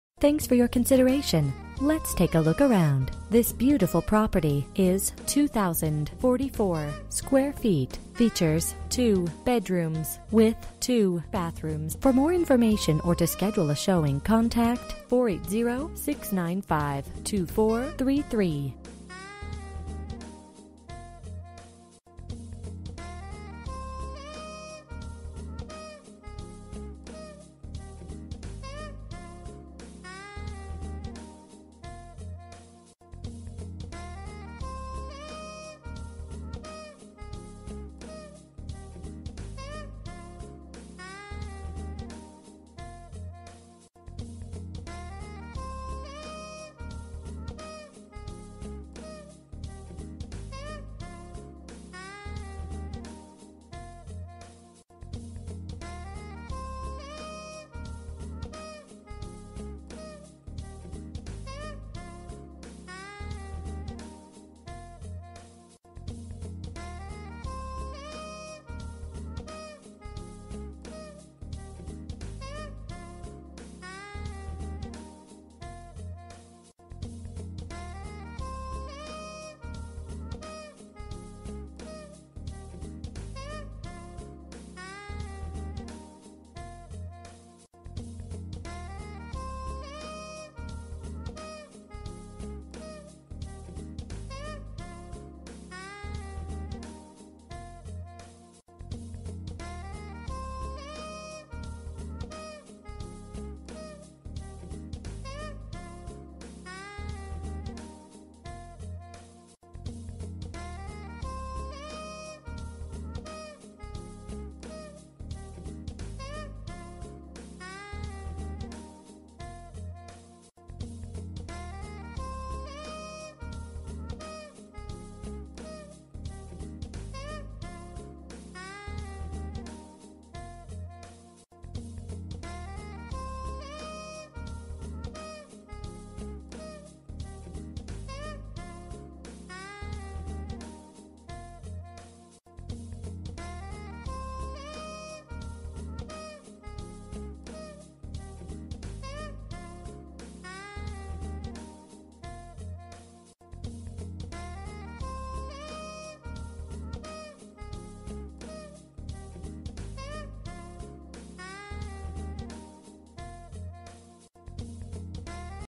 Residential for sale - 13577 E WETHERSFIELD Road, Scottsdale, AZ 85259
Views from every room! Relax and Enjoy your own ''Livable Elegance'' grand foyer, gourmet kitchen,family dining, great room featuring soaring wood ceilings and large windows with breath taking views.   Private backyard oasis with your very own spa, BBQ, very private with mountain views and lights.  Fabulous year round living, 2nd home or vacation rental.  A short walk to the Villas pool or make your way over to the large spa and tennis court... Come and see for yourself.

Bedrooms: 2
Bathrooms: 2 Bathrooms
Square Feet: 2044
Price: $499,000

For more information about this property, please contact Jean M Klien at or . You can also text 2576867 to 67299.